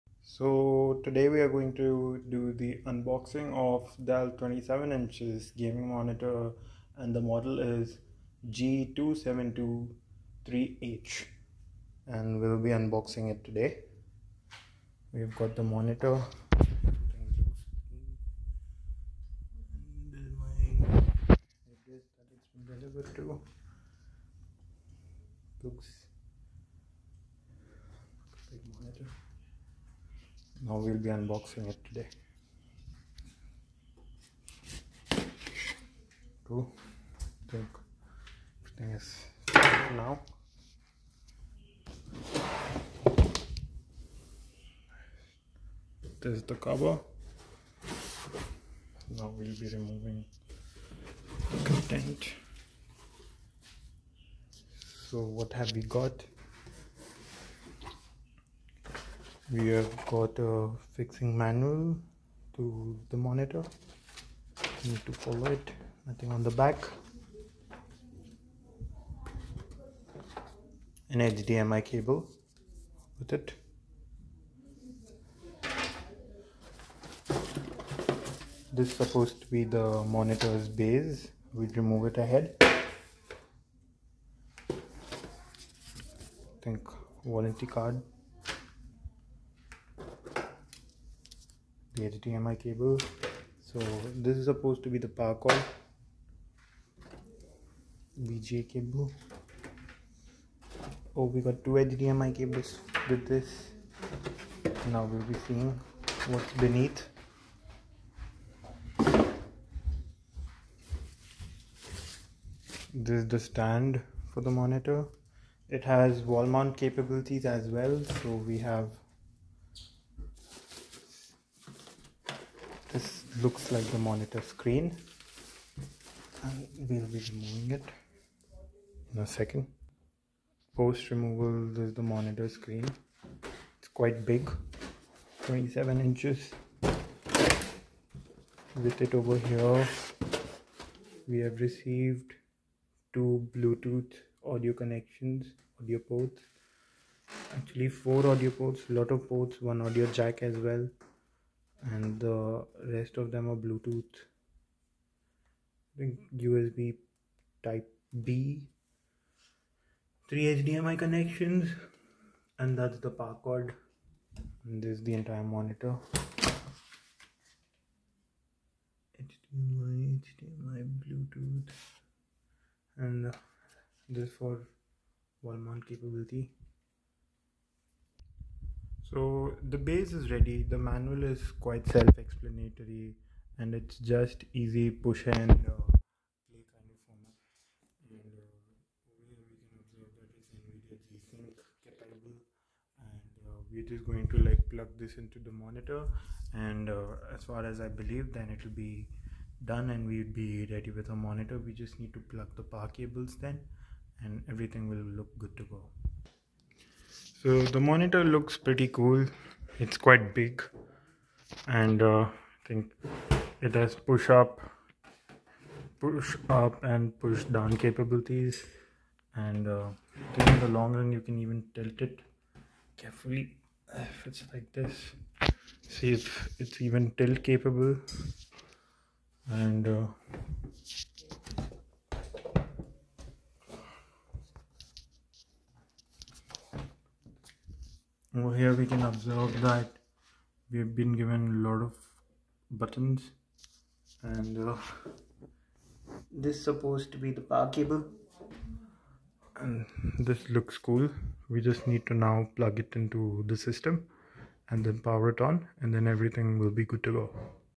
[Unboxing] Dell 27 Gaming Monitor G2723H
In The Box
What's Included
Monitor
Stand riser and base
Power cable (varies by country)
USB 3.2 Gen1 Type-A to Type-B upstream cable
1 x DisplayPort to DisplayPort cable
1 x HDMI Cable
Quick setup guide
Safety Information